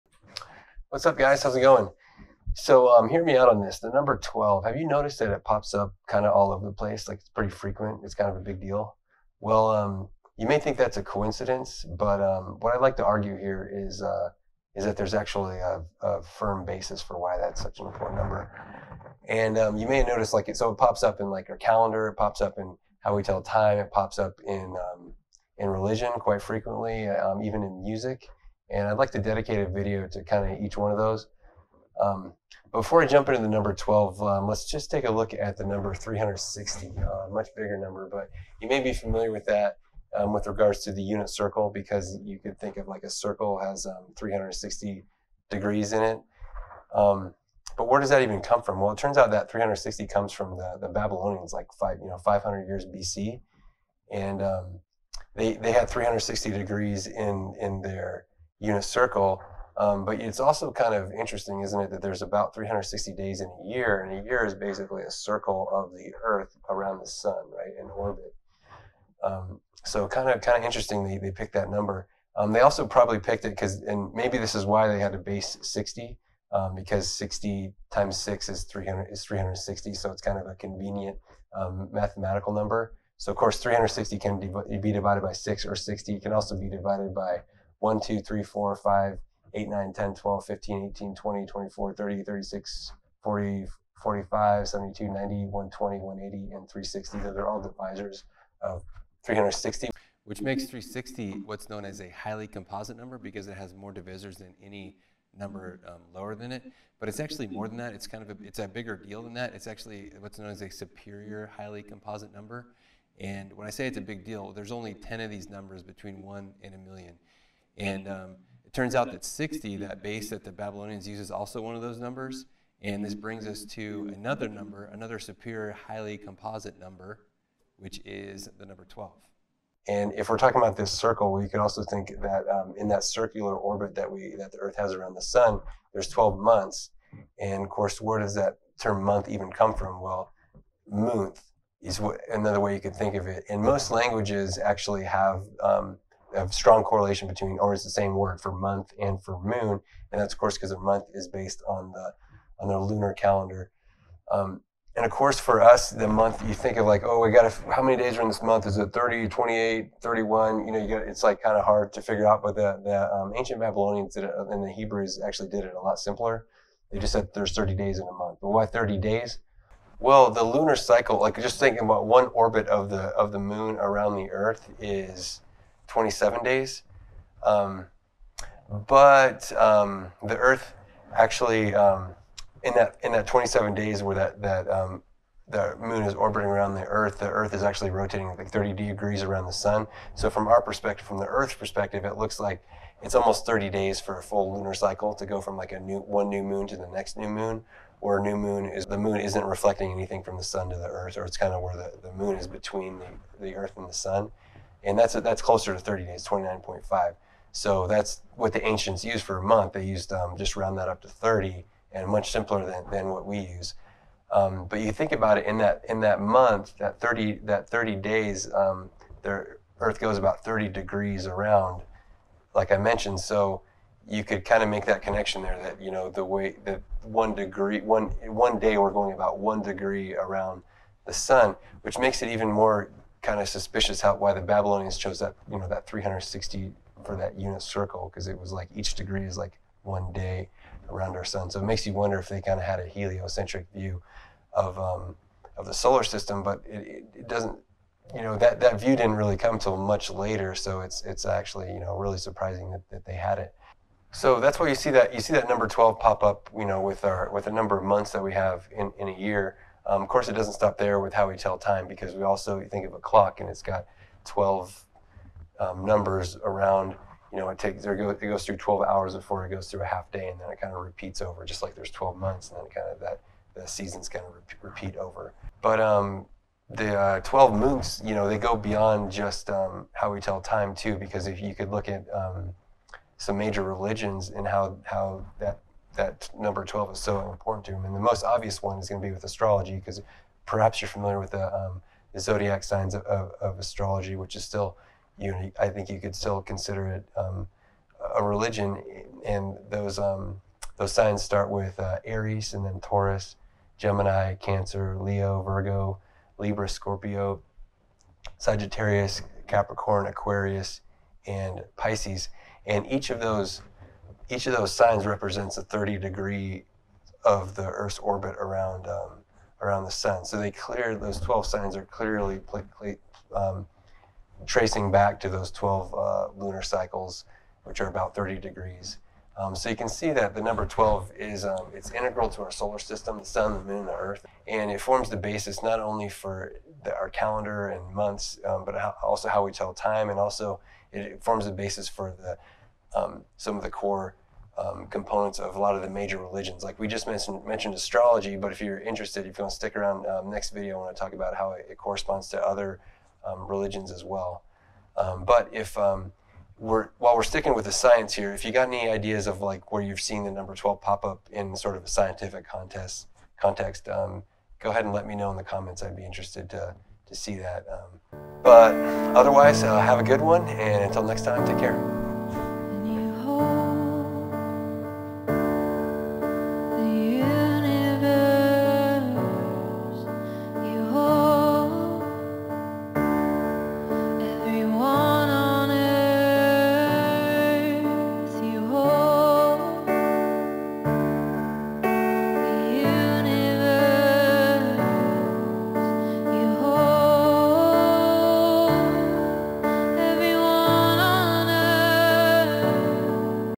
Why the number 12 is used to tell time
The number 12 shows up prominenty in our calendar, how we we tell time, several major religions, and even in music.
This first video is on the science behind it, The second one regarding its usage in religions And the third it's usage in music, which gets a little more mathy And what I'd like to argue is that this number showing up is not a coincidence There is a reason behind it, Before we get to the importance of the number 12, let’s start with a bigger number, 360 A number you’re probably familiar with, with regards to the unit circle, represented by 360 degrees around the circle.

But why 360 degrees in a circle, well, The ancient Babylonians’ used this and they also considered there to be 360 days in a year, just like the ancient Hebrews did. It’s very interesting they chose this isnt’ it? Because we know that a year is based on earths full orbit, or circle if you will, around the sun. 360 days (off by 5 technically) then the seasons repeat, but it doesn’t seem that our heliocentric understanding of the solar system came until much, much later than the Babylonians, so it’s very interesting they chose this number of days in a year to represent the number of degrees around the unit circle. The Babylonians also used a sort of base 60 number system and 360 was 6x60’s, so it’s a very mathematically convenient number.

So 360 can be divided evenly by 60, or 6, or by many other numbers. It is in fact regarded as a ‘highly composite number’ in that it has more divisors than any smaller number 1,2,3,4,5,6,8,9,10,12,15,18,20,24,30,36,40,45,60,72,90,120,180 and 360 All divisors of 360, lots of them One of those very important divisors of 360 is 12, which is also a ‘superior highly composite number’, in that it has more divisors than any number smaller than it. 12 is of course important in the context of the year, because there are 12 months in a year.

And where does the definition of month come from, well, think of the origin as ‘moonth’. This is not just true in English but the word for month in Mandarin and many other, if not most, languages comes from their terms for the moon as well. English Moon Moonth Mandarin 月 (yue) 月 (yue) Indonesian Bulan Bulan Turkish Ay ay So we have 12 months in a year in our calendar, and the ancient Babylonian and Hebrew calendars did too, except their months were all 30 days long.

Why 30, well, the moon orbits the earth in a little over 27 days. However, in that time it takes to orbit, the earth has gone about 30 degrees around the sun, so the moon needs to orbit a little bit more than one full rotation to go back to a new moon from the earth’s perspective. The new moon, by the way, happens when viewed from the earth, the moon is not reflecting any light from the sun, so it's basically when the moon falls between the earth and sun So you may notice that if in 30 days, the earth goes about 30 degrees around the sun, then each day the earth moves about 1 degree around the sun, with 12 months of 30 days giving 360 days in a year, each day like the earth moving (about) a degree. So the Babylonians choice of 360 degrees in a circle, after which the circle repeats, was quite ingenious.

And our use of the number 12 in keeping time doesn’t stop there of course, We use a circular clock to keep track of 12 hours as the hour hand completes one revolution in a half-day. So, we really like this number 12 when it comes to time and circles, and it shows. But these 12 ‘moonths’, if you will, also give rise to elements of human activity beyond just keeping track of time. Consider the Zodiac signs of astrology, which can be traced back all the way to the same Babylonians discussed earlier, and still constitute a sort of religion of astrology today. Perhaps you're familiar with the 12 Zodiac signs in Astrology, which are: Aries Taurus Gemini Cancer Leo Virgo Libra Scorpio Sagittarius Capricorn Aquarius, and Pisces And each represent a span of 30 degrees, or about a month, of the earth’s orbit around the sun.

So the prominence of the 12 signs in this religion clearly trace to the 12 lunar cycles in a year, So you can see that the 12 number is integral to the relation of the sun, moon and earth, which forms the basis for it’s importance in other areas of human existence as well, areas like religion, which is what we’ll look into in the next video. So check that out if you're interested.

But otherwise, have a good one, and take care.